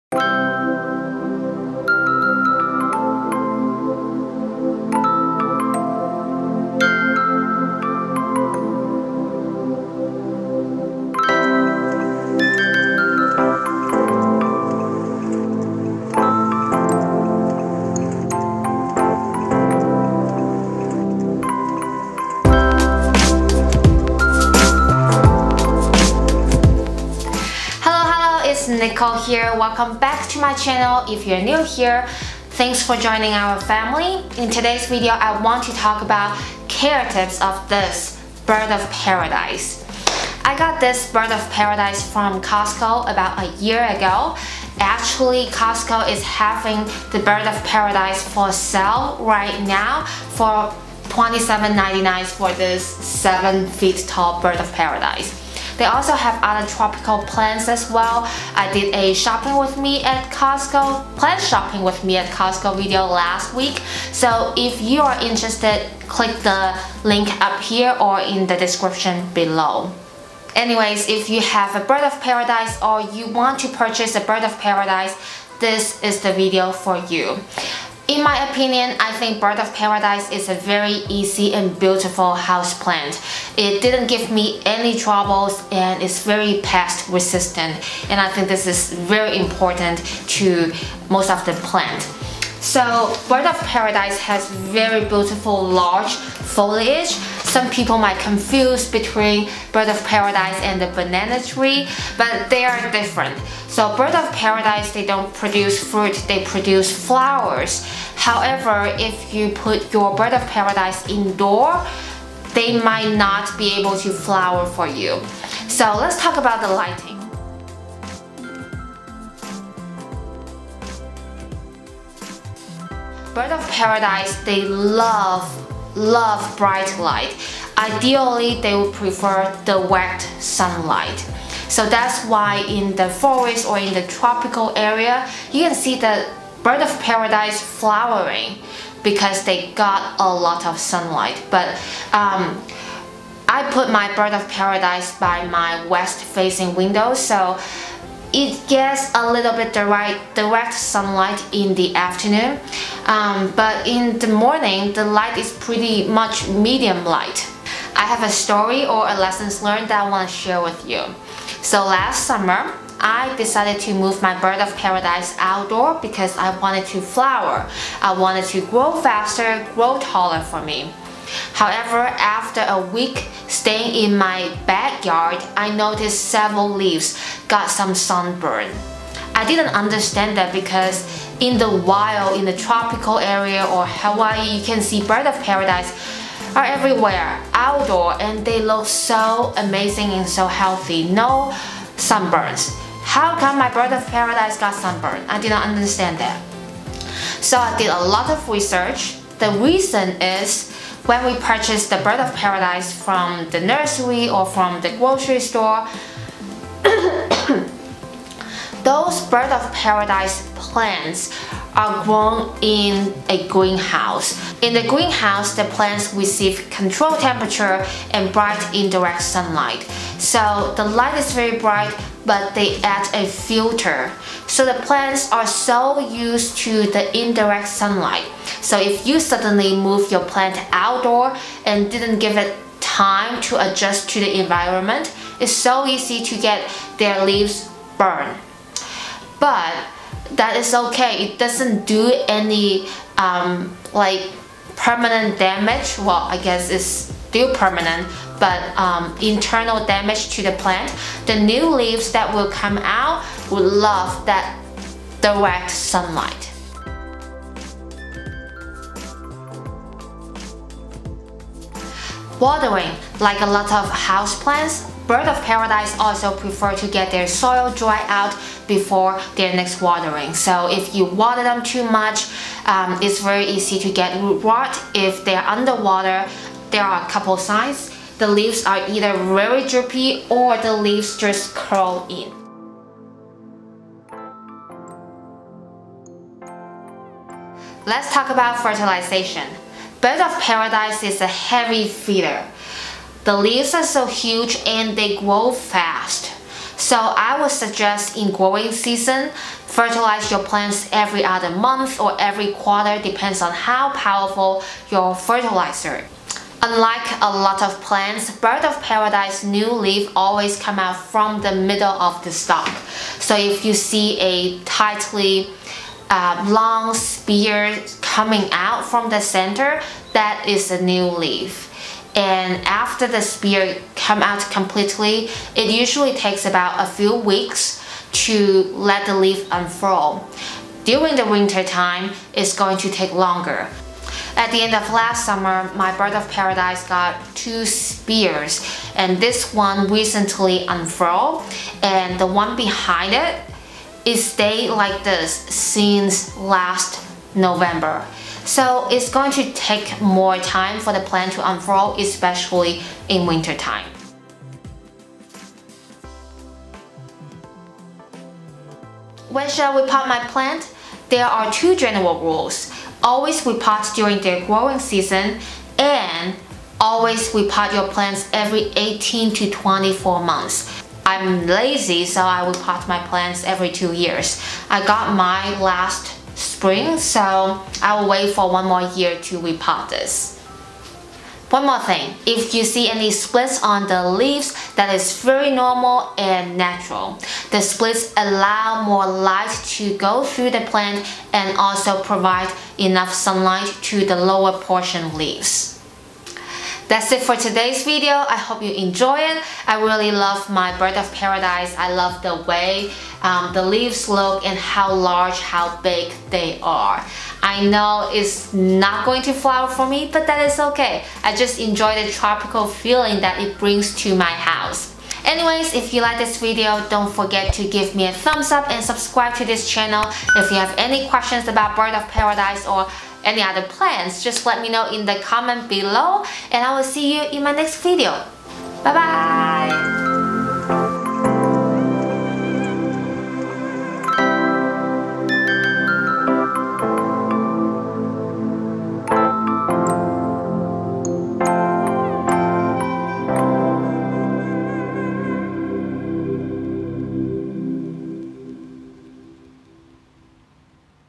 What’s Wrong With Your Bird of Paradise? | BOP Care Tips and Guide
Let’s talk about Bird of Paradise (BOP)! BOP is such a beautiful tropical plant. Like other houseplants, they will get sick if they don’t receive proper care. Today, I’m going to share some common issues with BOP and ways to avoid them.

Btw, Costco currently (March, 2020) has a lot of BOP and other tropical plants for $27.99